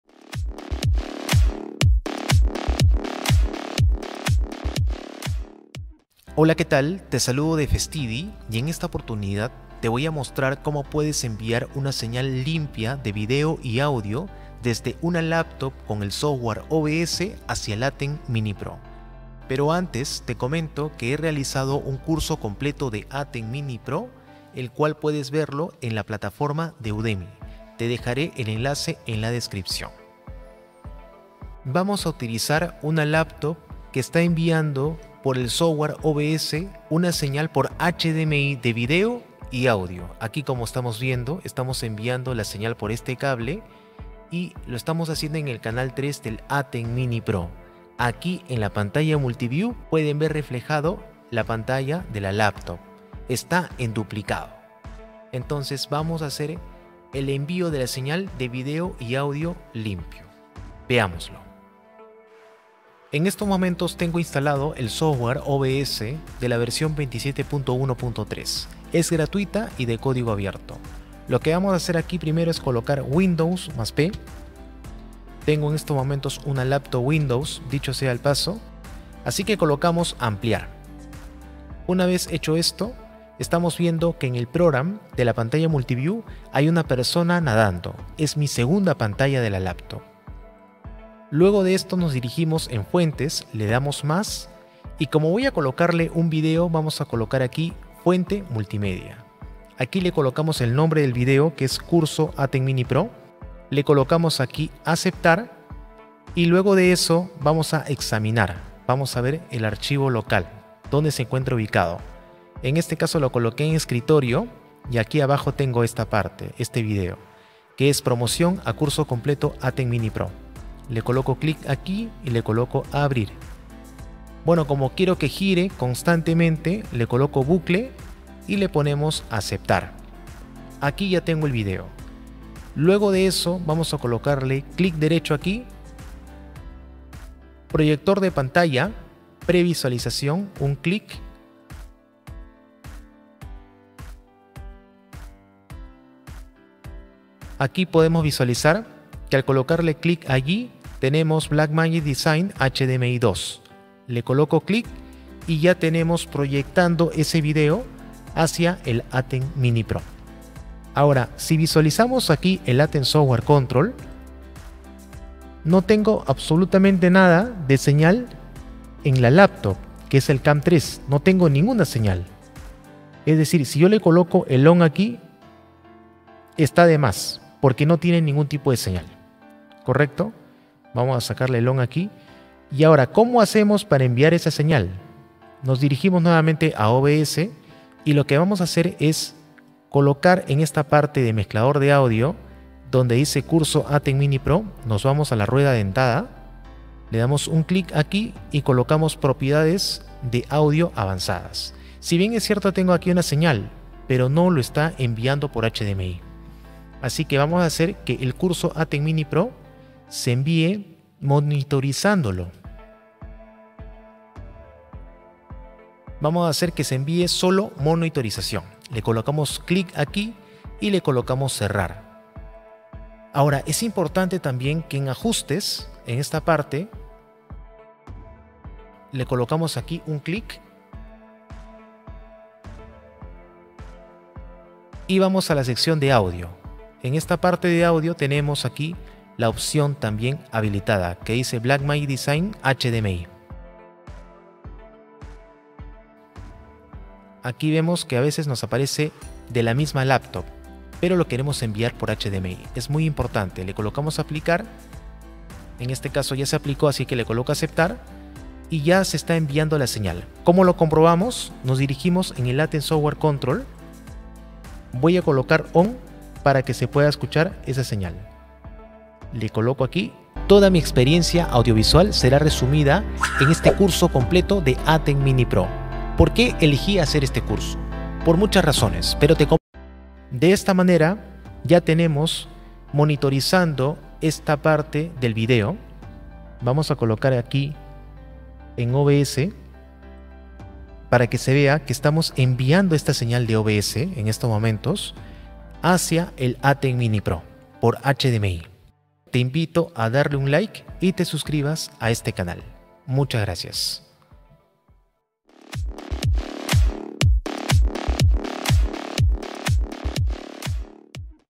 🎥🎤 Enviar video de OBS al ATEM Mini Pro a través de cable HDMI
Cuando necesitamos tener un video limpio en nuestro ATEM Mini Pro, podemos utilizar el Software OBS para poder hacerlo.

Aquí te explico las conexiones que estoy realizando y cómo configuro el OBS para poder obtener señal de video y audio.

🎥Live Streaming ♬Producción Audiovisual
🎤Eventos Corporativos (Lima - Perú)